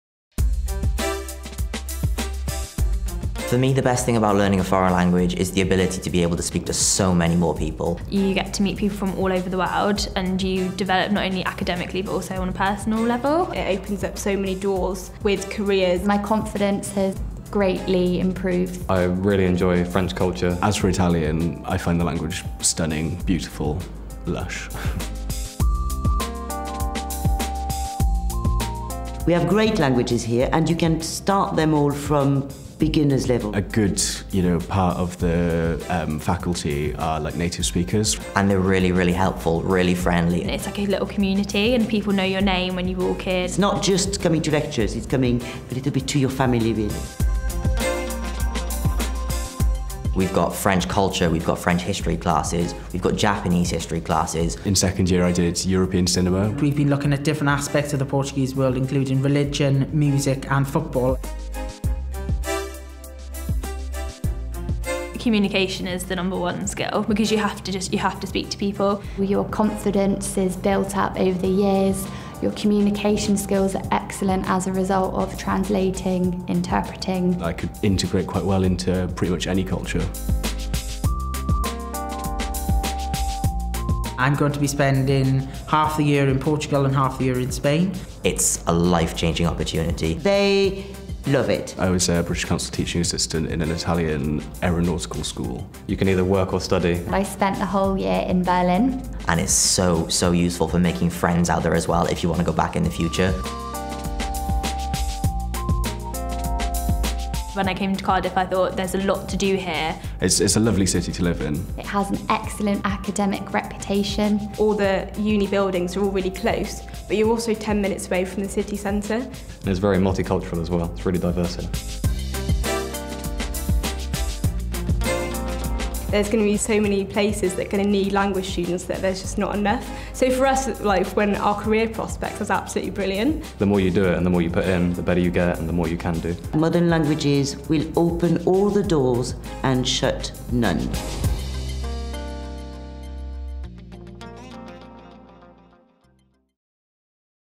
Studying Modern Languages at Cardiff University
Our students talk about their experience of studying at the School of Modern Languages in Cardiff.